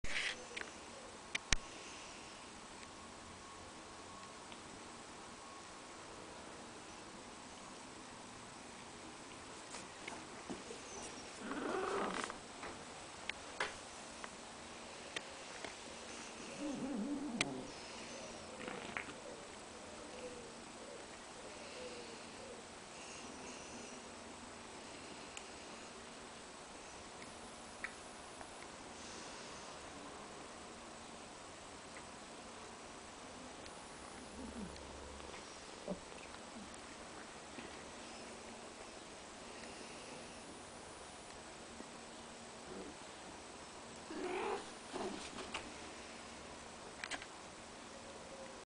La mamá gata NALA dormida y KIARA amamanta tranquilamente....
jueves por la mña , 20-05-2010.